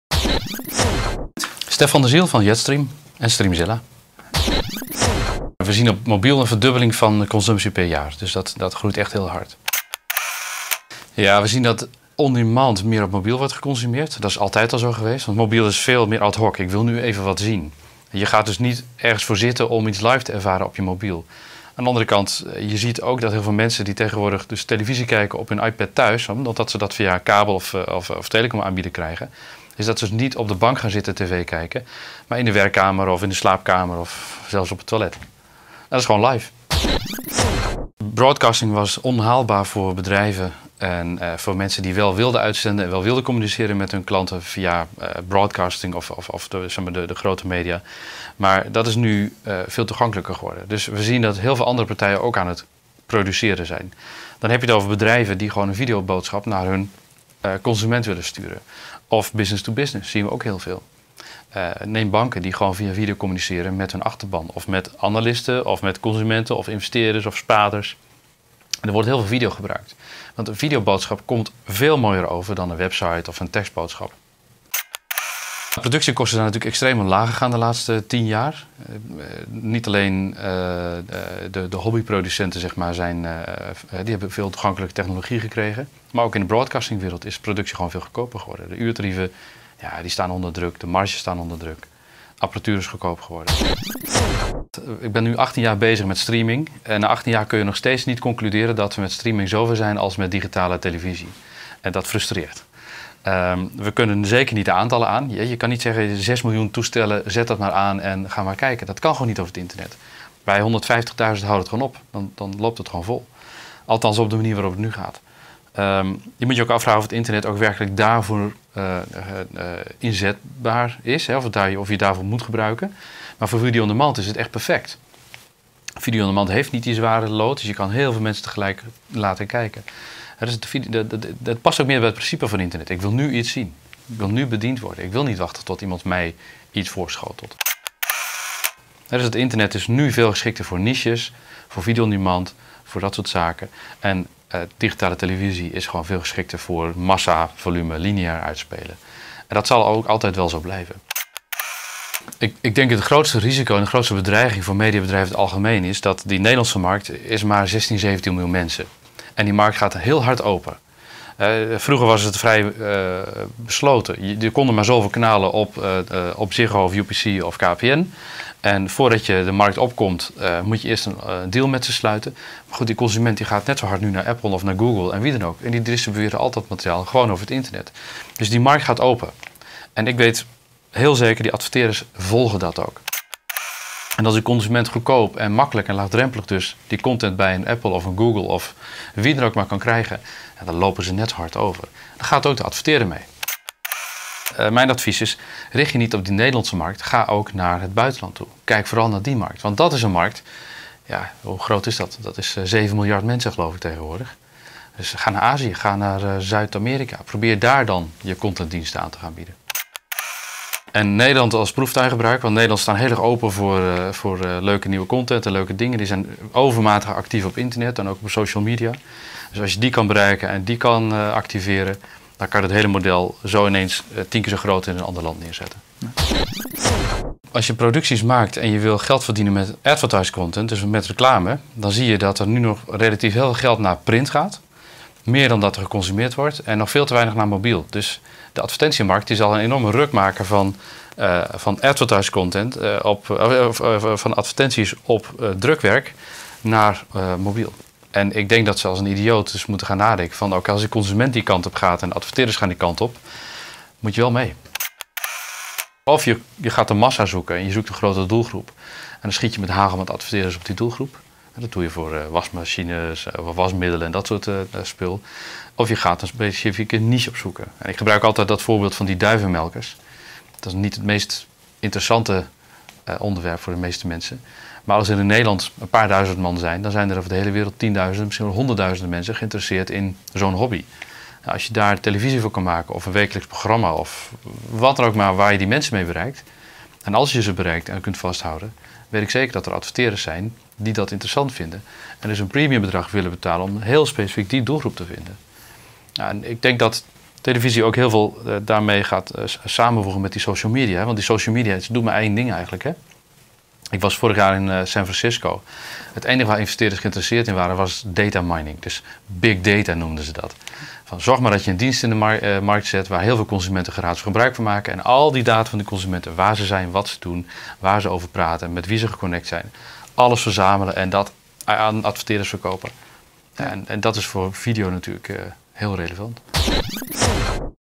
Stef van der Ziel (Jetstream): "Kies een niche en ga internationaal"
Stef van der Ziel heeft met zijn bedrijven Jetstream en Streamzilla al 18 jaar ervaring met streaming van audio en video. Zijn bedrijf timmert internationaal aan de weg. De groei die hij bij online video signaleert is explosief, met name op de mobiel, maar de techniek blijft ook een beperkende factor. Hij zet zijn visie op een rij in de eerste van een serie interviews ter gelegenheid van het Mediapark Jaarcongres 2013 in Hilversum. Fast Moving Targets is daar van de partij!

"De videoconsumptie op de mobiel verdubbelt per jaar. On demand is daarbij het meest populair vanwege het ad hoc karakter; nu even iets specifieks bekijken. Tegelijk gebruiken mensen hun iPad thuis om alles te bekijken, ook live tv op de wc."

"Media wordt voor iedereen toegankelijk, daardoor gaan andere dan de klassieke partijen produceren. Bijvoorbeeld banken gebruiken video om met hun achterban te communiceren. Een videoboodschap komt mooier over dan een tekstboodschap. De productiekosten zijn extreem omlaag gegaan de laatste tien jaar. Voor amateurs en voor profs."

"Na 18 jaar ervaring met streaming kan ik nog steeds niet zeggen dat we met streaming even ver zijn als met digitale televisie. En dat frustreert. Bij 150.000 tegelijk kijkende mensen loopt het vol. Maar voor video on demand is het perfect. Dat past ook beter bij het internet: 'ik wil nu iets zien, ik wil nu bediend worden'. Het internet is geschikter voor niches en voor video on demand. Digitale televisie is geschikter voor massa, voor volume. Voor lineair uitspelen. Dat zal ook altijd wel zo blijven."

"De Nederlandse markt is maar 16, 17 miljoen mensen. En die markt gaat heel hard open. Buitenlandse spelers komen hier naartoe. Of het nu Apple, Google of Netflix is. De markt gaat open en de adverteerders volgen die ontwikkeling. Mijn advies is: richt je niet op de Nederlandse markt, maar kijk naar de buitenlandse mogelijkheden. Ga naar Azië of Zuid-Amerika. Probeer daar je contentdiensten aan te bieden en gebruik Nederland als proeftuin."

"In de reclame gaat nu relatief veel geld naar print en nog veel te weinig naar mobiel. Die ruk gaat nog gemaakt worden. Als de consument naar de mobiel gaat en de adverteerder gaat naar de mobiel, moet je als producent wel mee. Of je kiest voor de massa en schiet als adverteerder op een doelgroep, of je zoekt een specifieke niche op."

"Ik gebruik altijd het voorbeeld van duivenmelkers. Als er in Nederland een paar duizend van zijn, zijn er in de hele wereld misschien wel honderdduizenden in zo'n hobby geïnteresseerd. Als je daar televisie voor kan maken, zijn er adverteerders die een premium bedrag willen betalen om specifiek die doelgroep te bedienen."